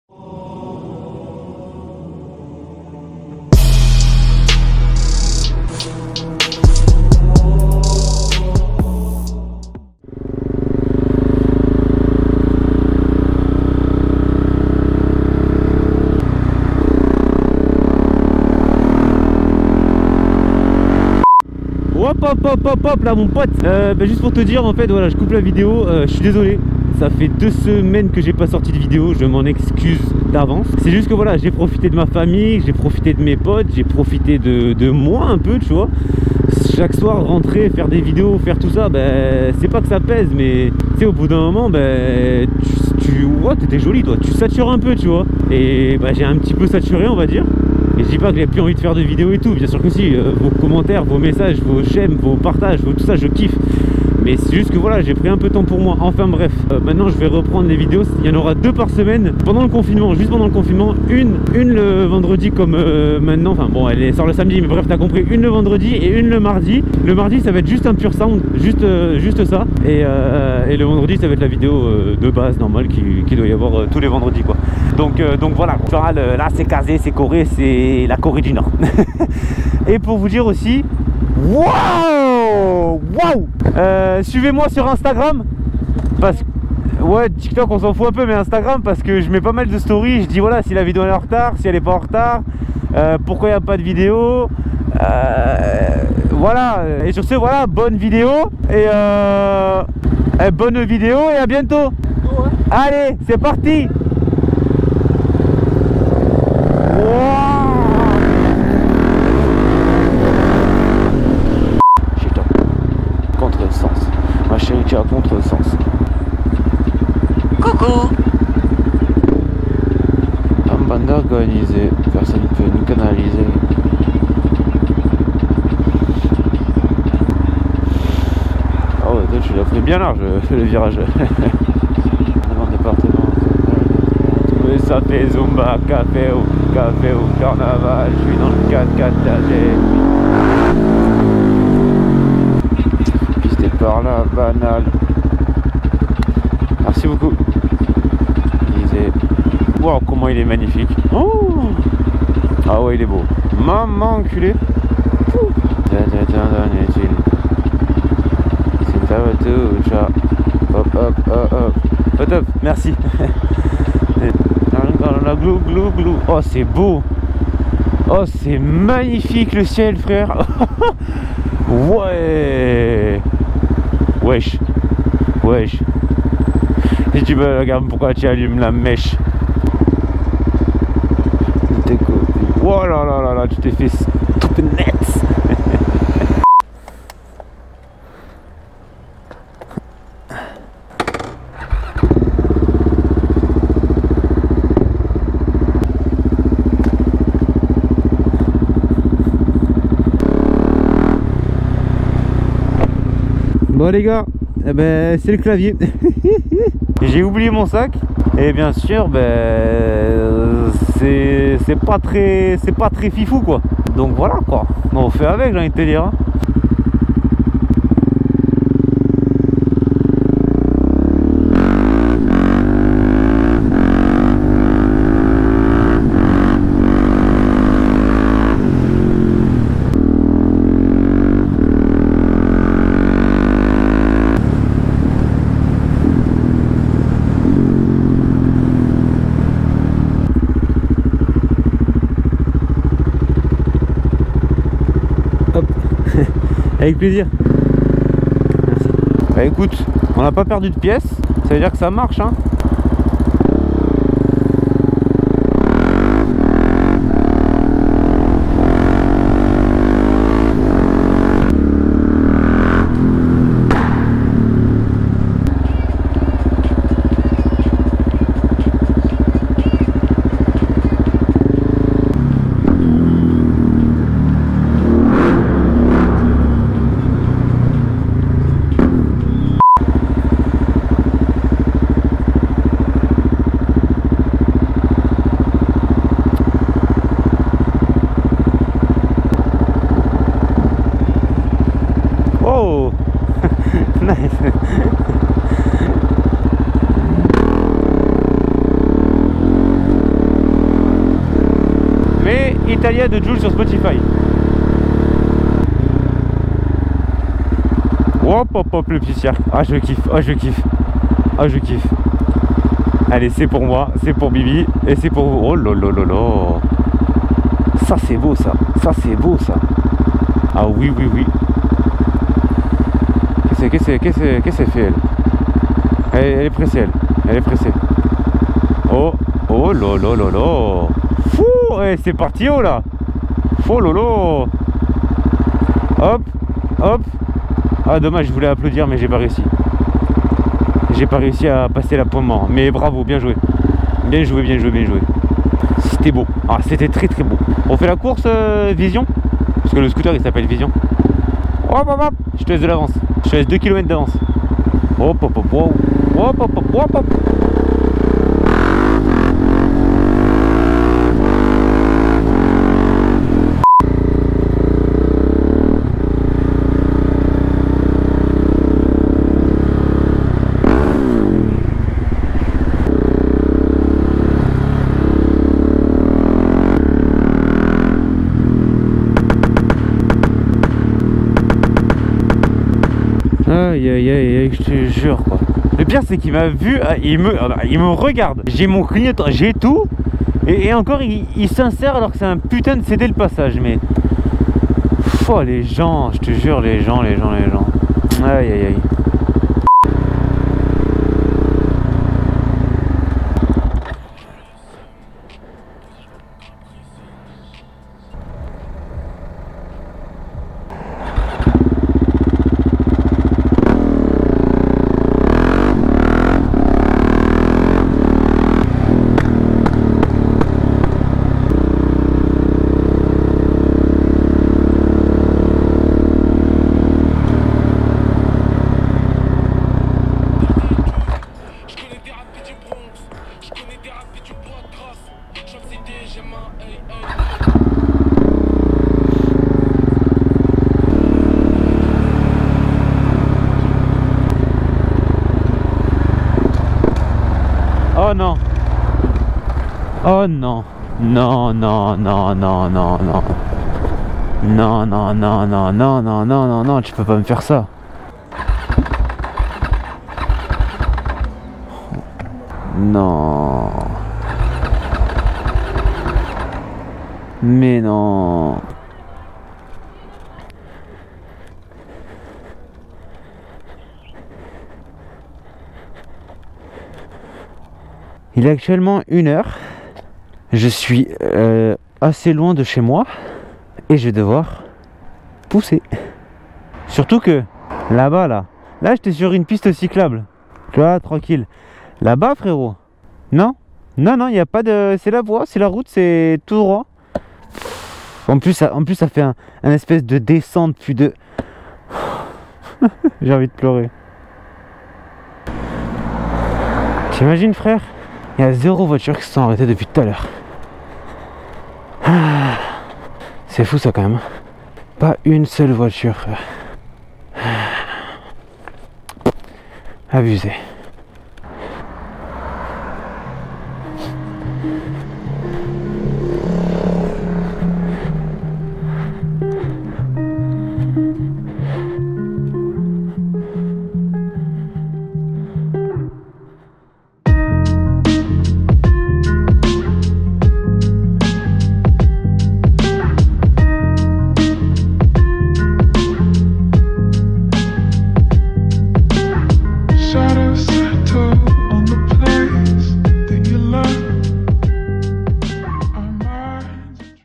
PAIKA EXPRESS - EN WHEELING AVEC UN CLAVIER 😂
Aujourd'hui on se retrouve pour une nouvelle vidéo, j’espère qu'elle vous plaira et je suis sur INSTAGRAM aussi, où je poste des photos donc allez voir ça 😉

MOTO :
- KTM DUKE 690 de 2017
- Poignée KTM
- Pot AKRAPOVIC

CAMERA :
- GoPro Hero 5/7
- Iphone X